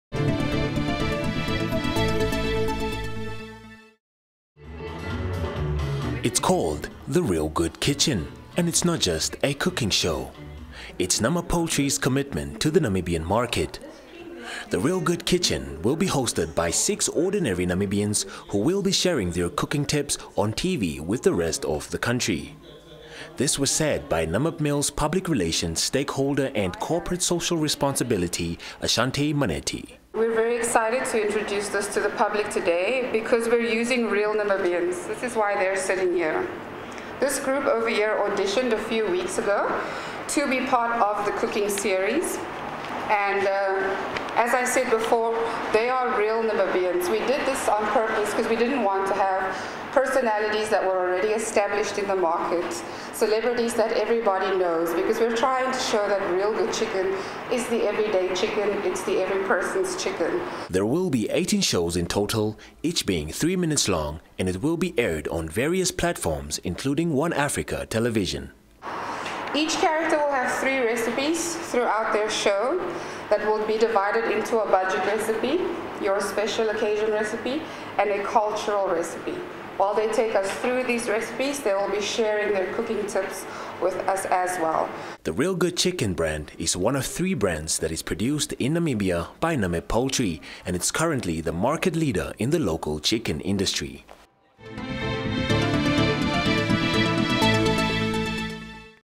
Real good chicken with the The Real Good Kitchen. By: Isai Sipunga
It's called "The Real Good Kitchen" and it's not just a cooking show, it's Namib Poultry's commitment to the Namibian market. This was said by Namib Mills Public Relations, Stakeholder and Corporate Social Responsibility Manager, Ashante Manetti. There will be 18 shows in total, each being 3 minutes long, and it will be aired on various platforms, including One Africa Television. The Real Good Chicken brand is one of three brands that is produced in Namibia by Namib Poultry and its currently the market leader in the local chicken Industry.